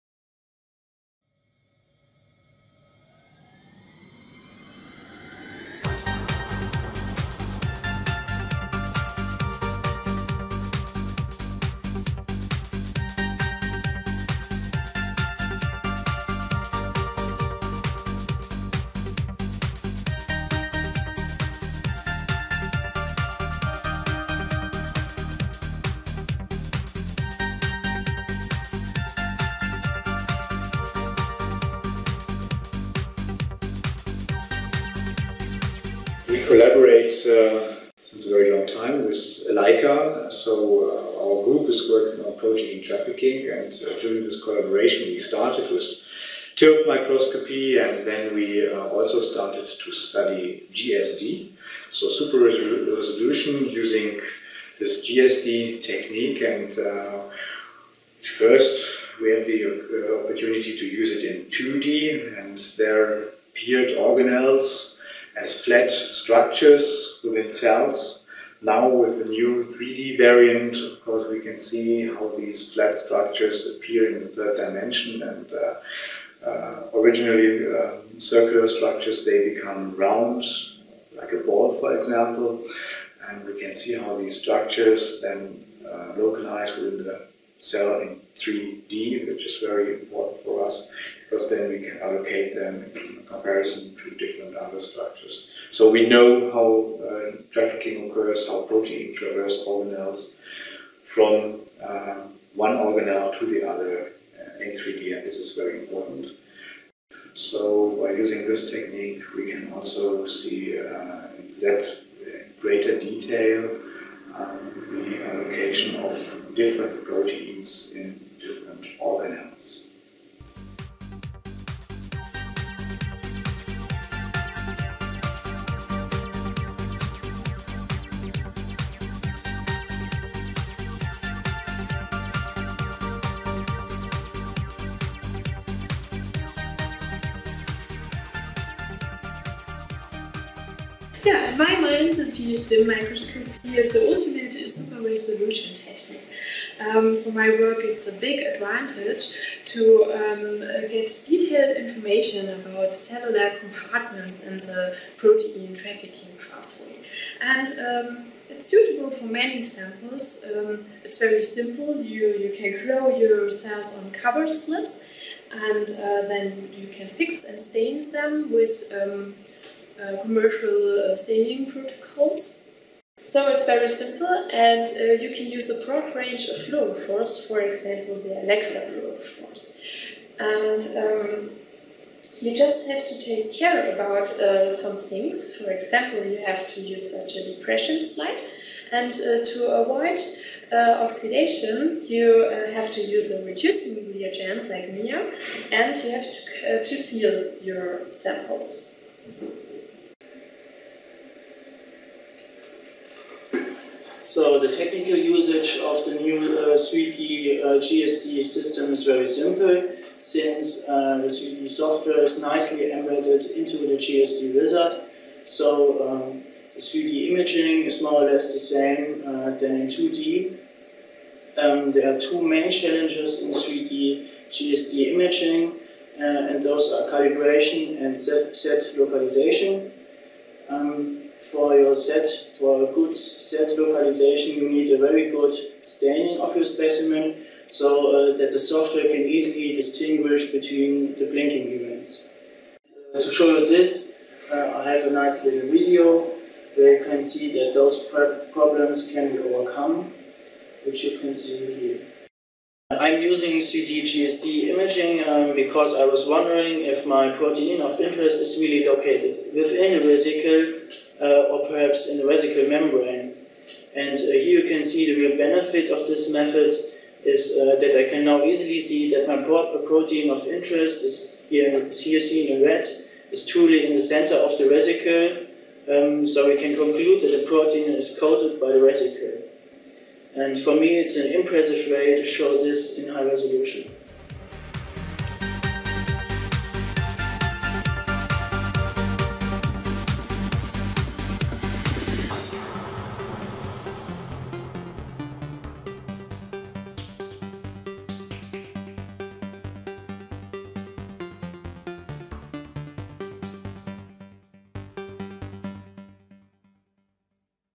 User experience with the Leica SR GSD 3D (2)
3D Localization Microscopy:  Prof. Ralf Jacob,  Sebastian Bänfer, Philipps University Marburg, Germany, and Tamara Straube, Leica Microsystems, who are experienced GSD users, talk about their experiences with the Leica SR GSD 3D widefield super-resolution microscope in cell biology.